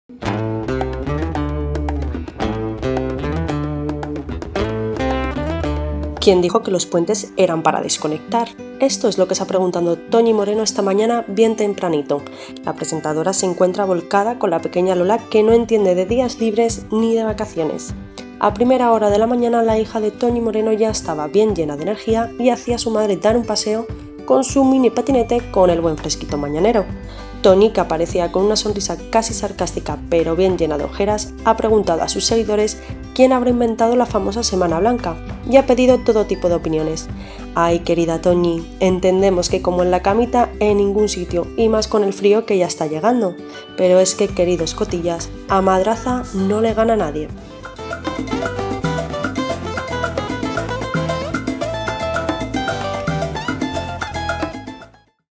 ♥️ Toñi Moreno y su divertido momento con su hija: ¡quién lo ha inventado! ➡︎ Cotilleo.es
♥️ Quien dijo que los puentes eran para desconectar. Esto es lo que se ha preguntado Toñi Moreno esta mañana bien tempranito. La presentadora se encuentra volcada con la pequeña Lola que no entiende de días libres ni de vacaciones. A primera hora de la mañana la hija de Toñi Moreno ya estaba bien llena de energía y hacía a su madre dar un paseo con su mini patinete con el buen fresquito mañanero. Toñi que aparecía con una sonrisa casi sarcástica pero bien llena de ojeras ha preguntado a sus seguidores quién habrá inventado la famosa semana blanca y ha pedido todo tipo de opiniones. Ay, querida Toñi, entendemos que como en la camita en ningún sitio y más con el frío que ya está llegando. Pero es que, queridos cotillas, a madraza no le gana nadie.